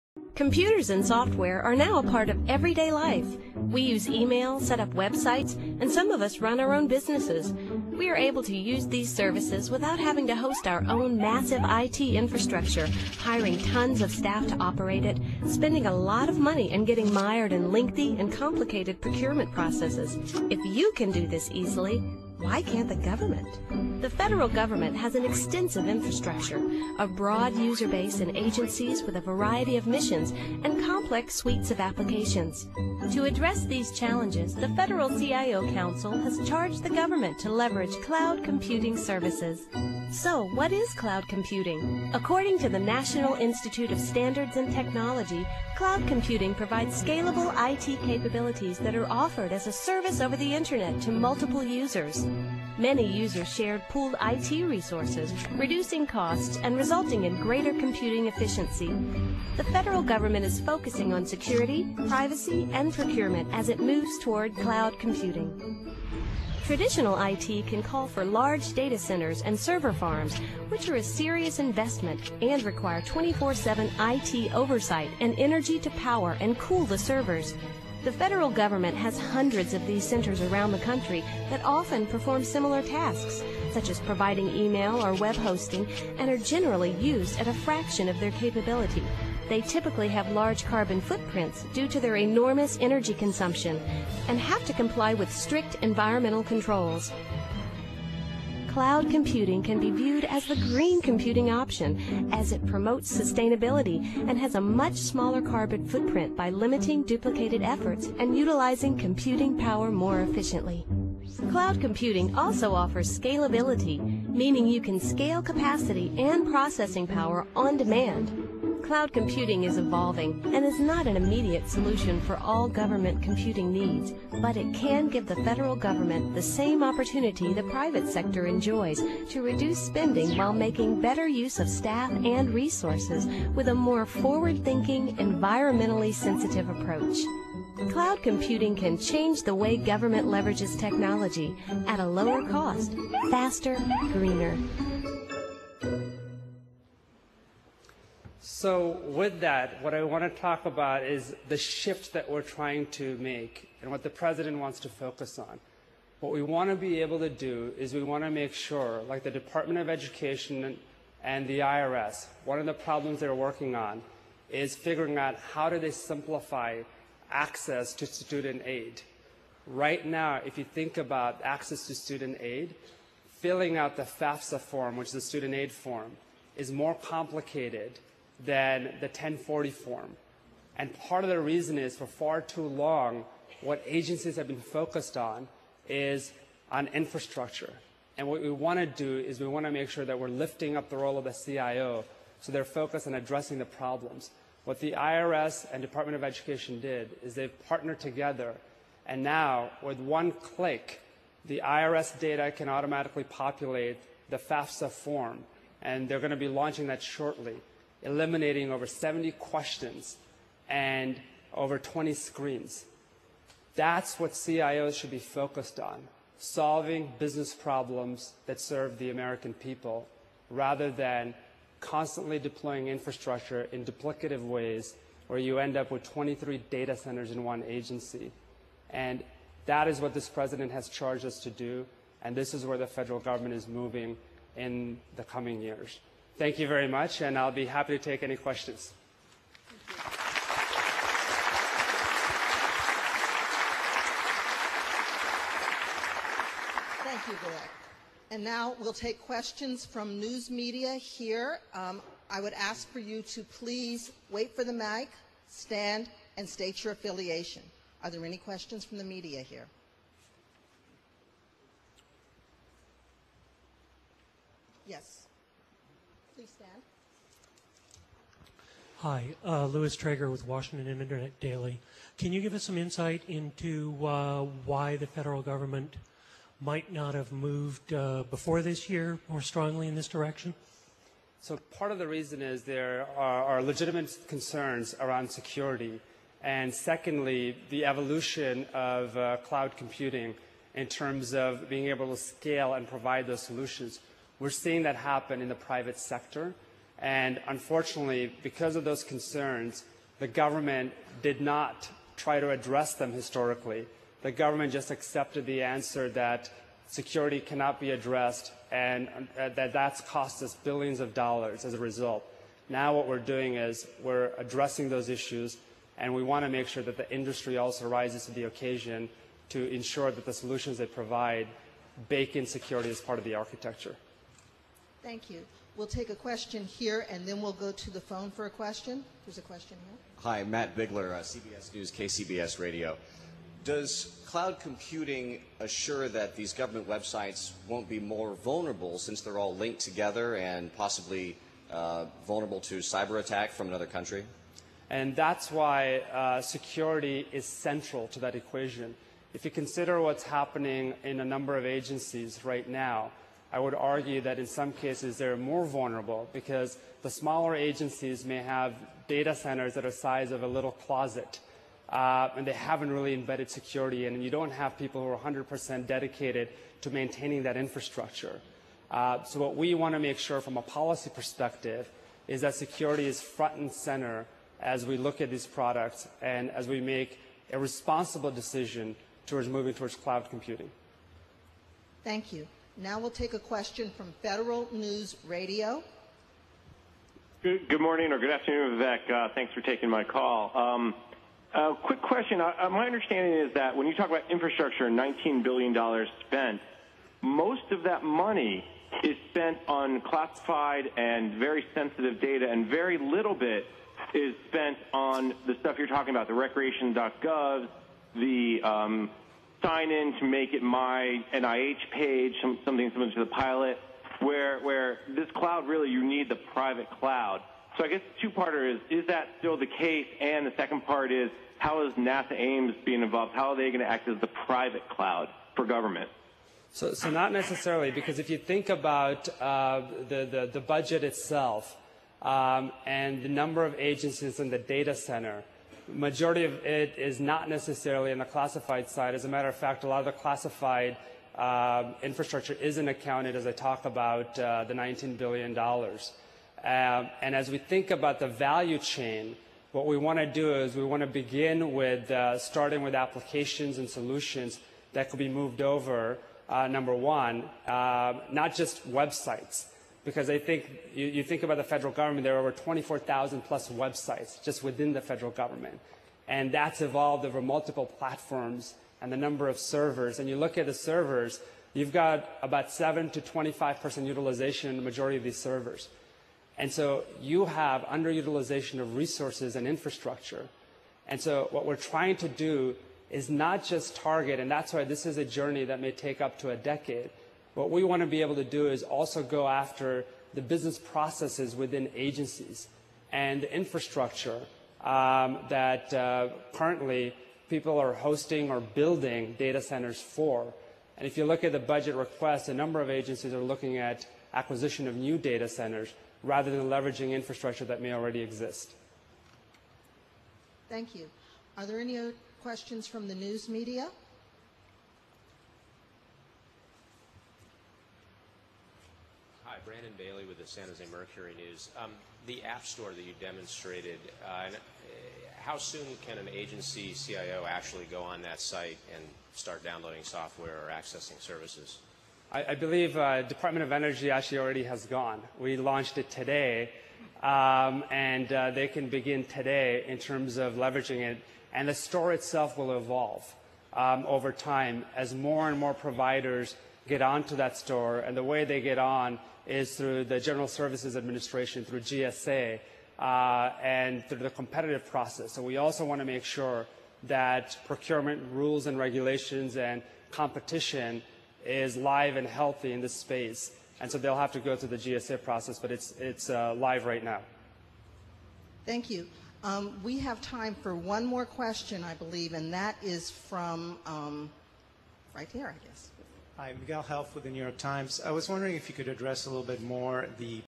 Vivek Kundra Cloud Computing Part 2/3
Vivek Kundra, Fed CIO spoke at AMES at NASA on US Govt Cloud Computing initiatives. This was broadcast by NASA TV.